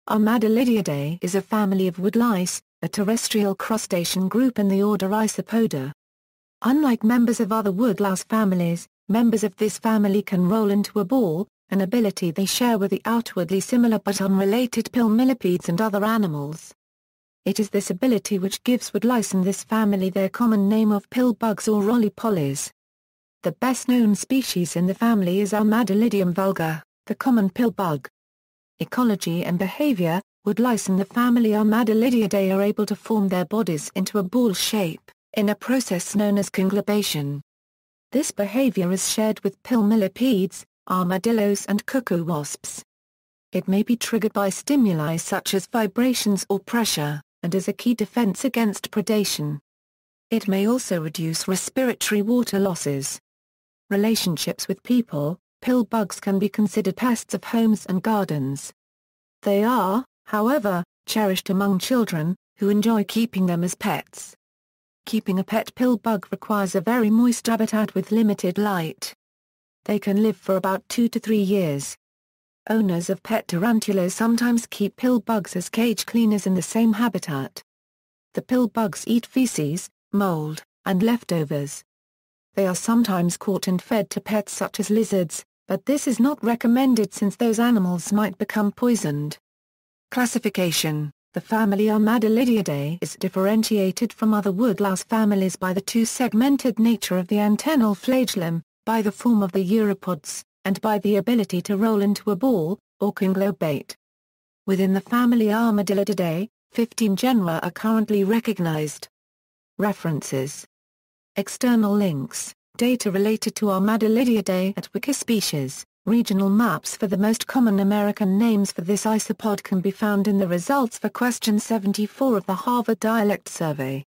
Armadillidiidae
Armadillidiidae is a family of woodlice, a terrestrial crustacean group in the order Isopoda. Unlike members of other woodlouse families, members of this family can roll into a ball, an ability they share with the outwardly similar but unrelated pill millipedes and other animals. It is this ability which gives woodlice in this family their common name of pill bugs or roly polies. The best known species in the family is Armadillidium vulgare, the common pill bug.